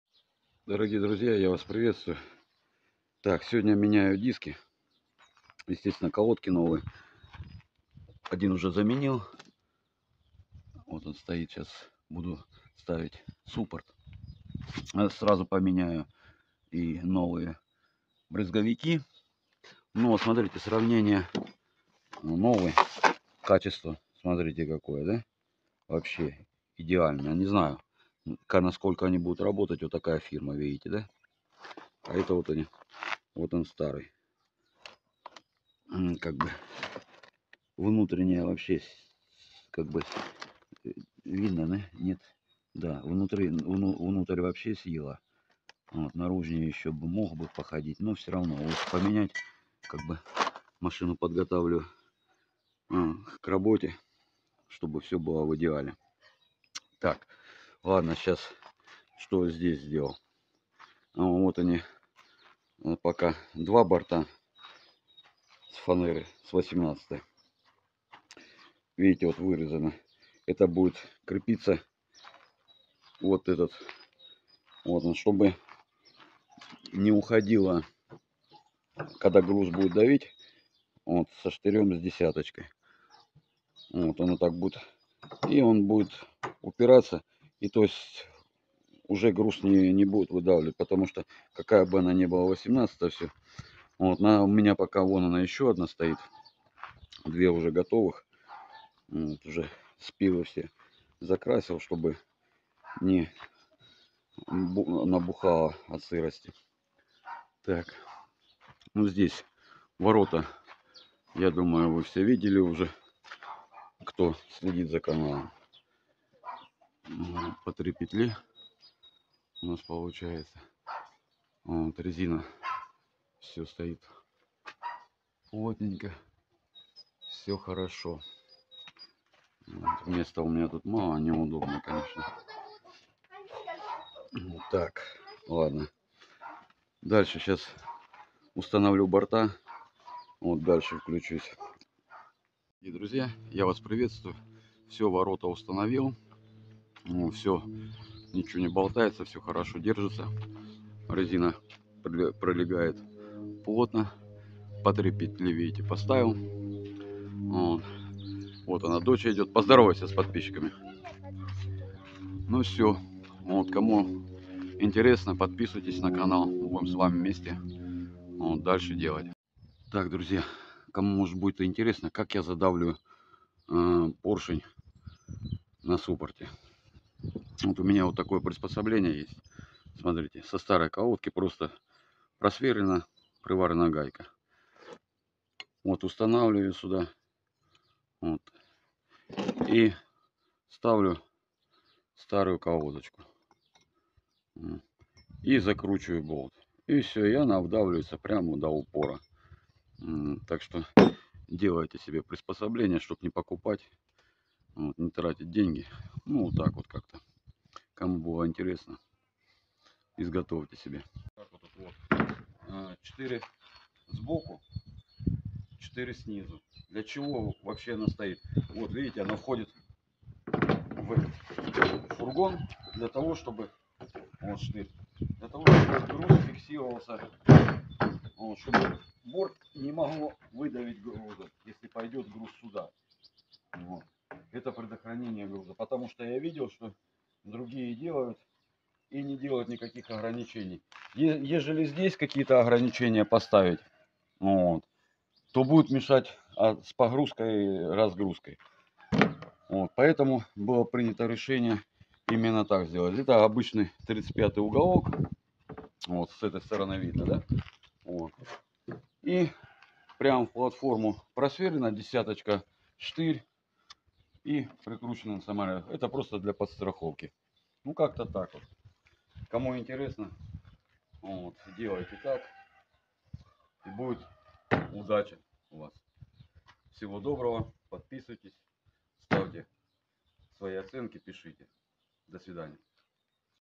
Удлинение Газели. Замена тормозных дисков.
Удлиняю Газель сам своимм руками. Сегодня поменял передние тормозные диски. Старые оригинальные проходили год. Посмотрю сколько проходят эти. Делаю автомобиль для работы. Работа на Газели. Посмотрим вместе результат. Влог. Болталка.